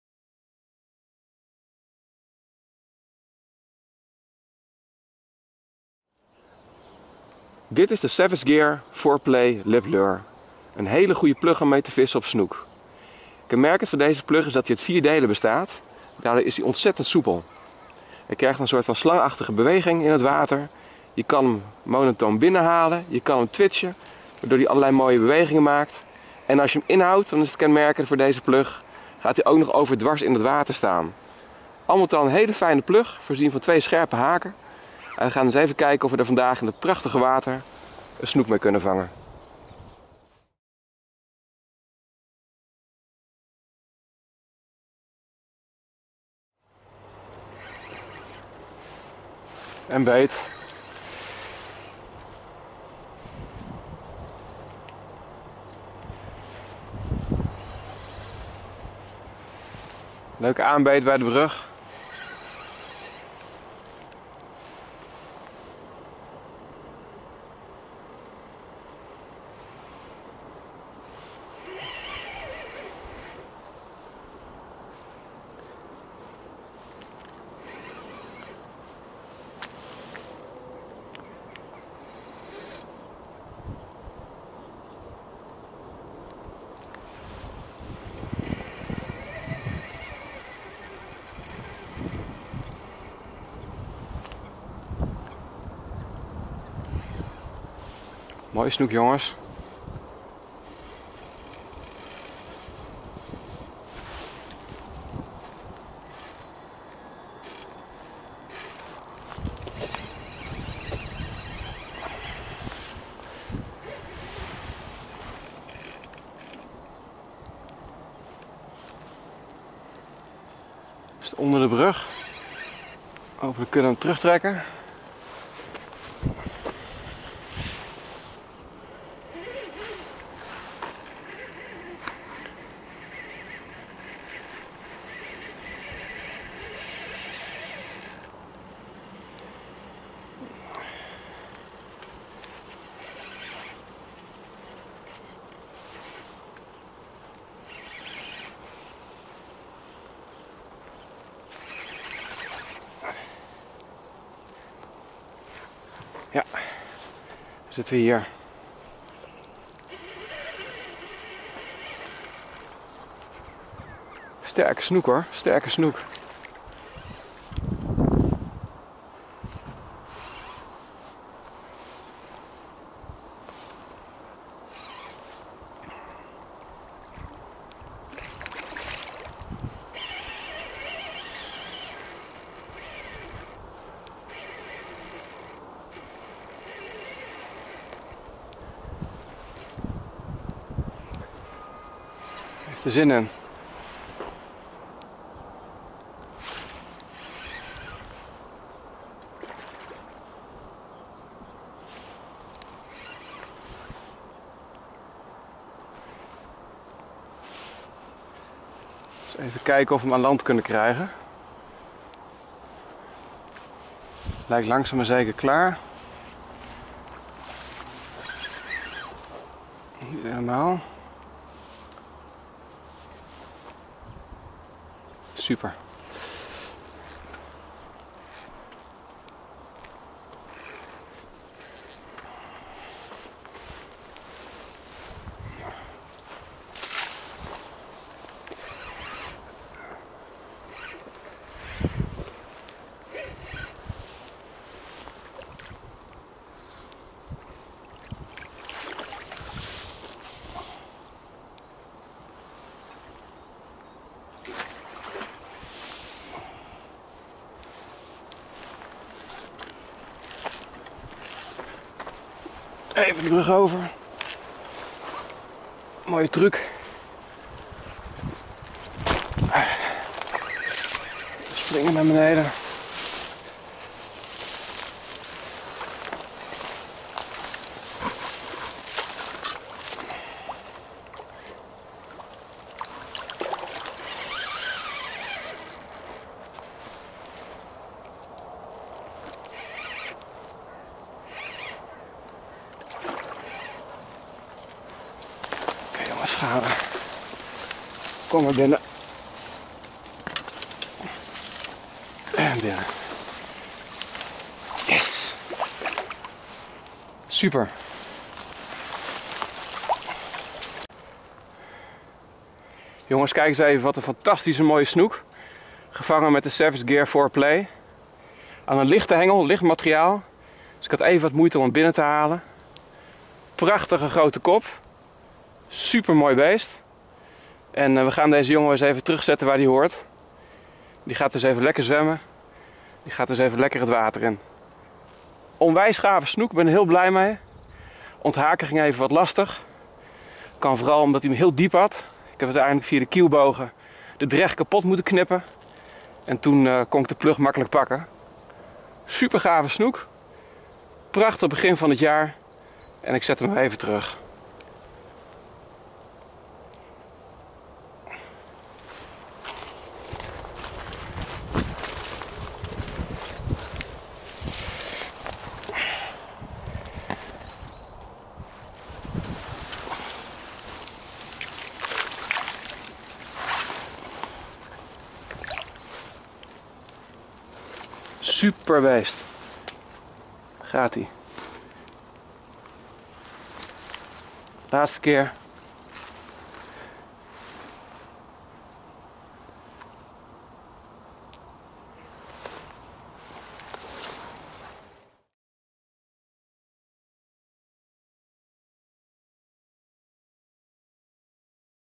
Snoek vissen Savage Gear 4Play 91 cm
Meer informatie over de Savage Gear 4play:
Meer informatie over Savage Gear:
Meer informatie over kunstaas pluggen:
Meer informatie over kunstaas:
De Savage Gear 4Play is bijzonder goede kunstaas plug om mee te vissen op snoek. In de video laten we je de 13 centimeter lange Savage Gear 4Play plug in volle actie zien. Na wat uitleg over de kracht van de Savage Gear 4Play plug zetten we hem op fraai water in. En dat werpt zijn vruchten af! Al staande op de brug wordt de Savage Gear 4Play gegrepen door een snoek van 91 centimeter. Na een spectaculaire dril van ruim 4 minuten wordt de snoek gevangen. De Savage Gear 4Play zat diep gehaakt maar via de kieuwbogen was het makkelijk om de dreggen door te knippen. Nadat de snoek is bewonderd zetten we hem weer netjes terug in het water.